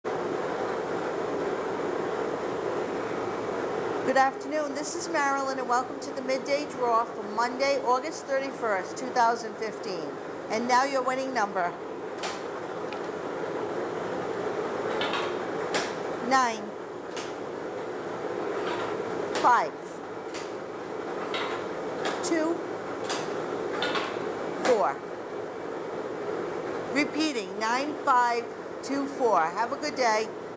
RI Lottery Midday Drawing 08/31/2015
Rhode Island Lottery's Midday Drawing for The Numbers on Monday, August 31, 2015.